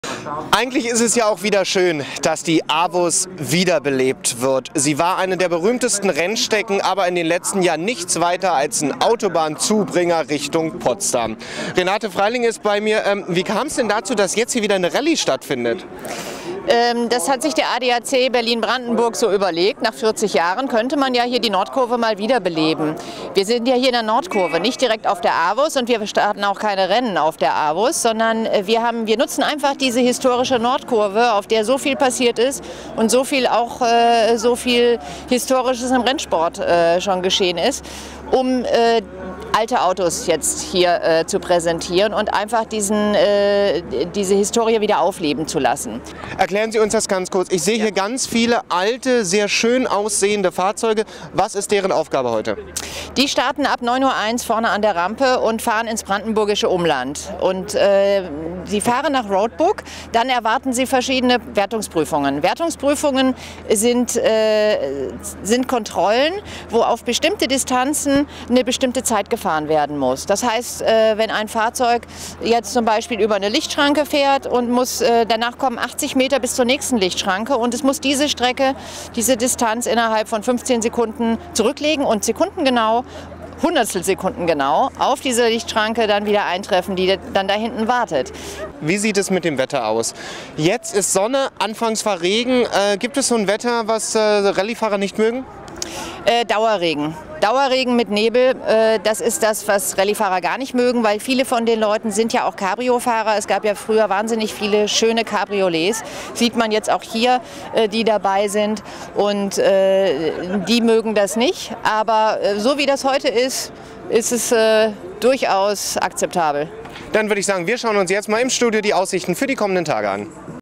TV BERLIN: ADAC Rallye AVUS Classic (Mit Heidi Hetzer) 2011
A TV BERLIN report at 2011's ADAC Rallye AVUS Classic, Berlin.

Amongst the classic car enthusiasts and drivers was Heidi Hetzer, born in 1937, and still the fastest female rally driver in Berlin today.

Hetzer competed in her first race on a Lambretta motor scooter when she was just 16 years old, and her longstanding career as has seen her secure more than 150 victories including completing the thousand-mile journey on the Mille Miglia, from Brescia to Rome and back, and taking part in the Monte Carlo Rally and the Düsseldorf-Shanghai Rally. The classic car driver took third place at both the Carrera Panamericana in Mexico and the Tour d'Europe in 1989. In 2004 Heidi carried the Olympic torch en route to Athens in 2004.

Although retired for many years, Heidi Hetzer still has a very active dream: to be on the starting line for the legendary Paris-Dakar Rally; the only rally she is yet to enter.

Reported by Sascha Berlin
Filmed by Jessica Van Lachterop
Edited by Sascha/Jessica